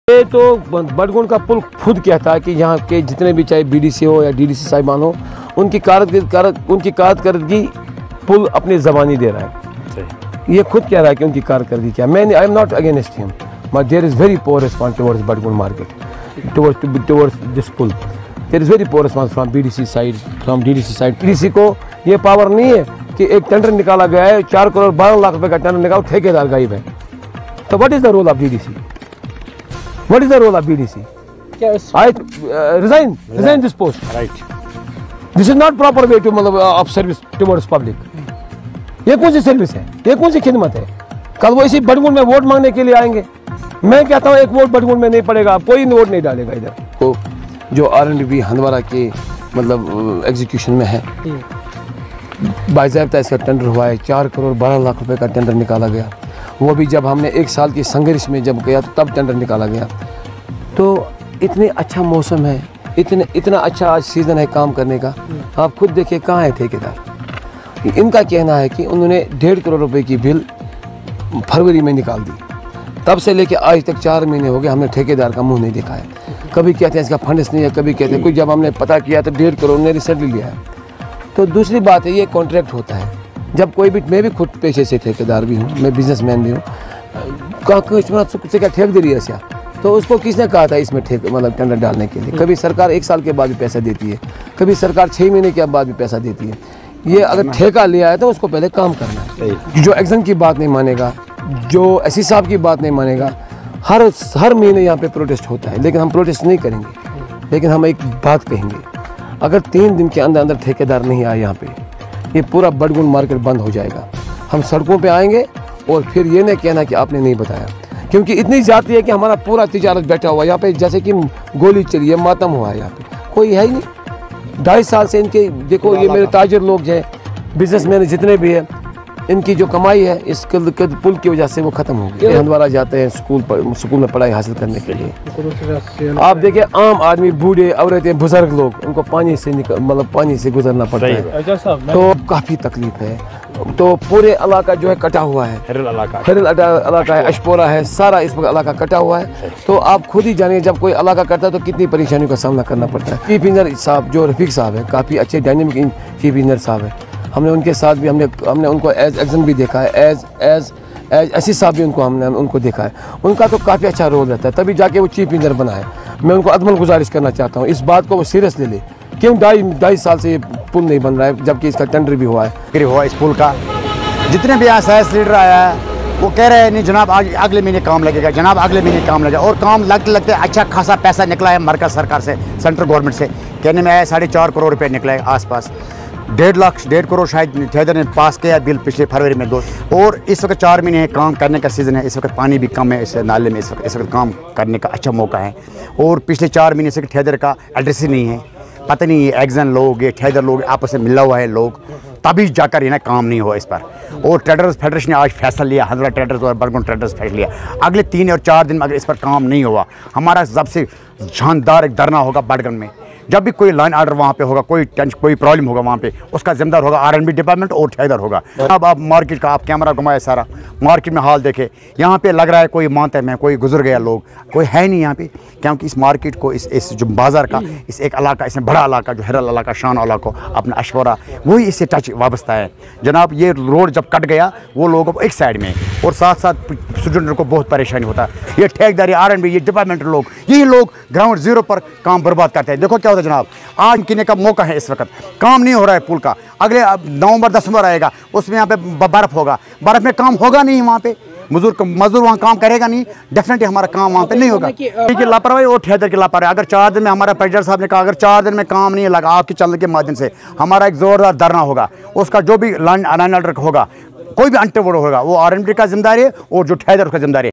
Damaged Batagund bridge awaits for completion
Handwara Trade President Seeks Restoration of Vital Batagund Bridge; Urges Authorities Not to Force Them Onto the Roads.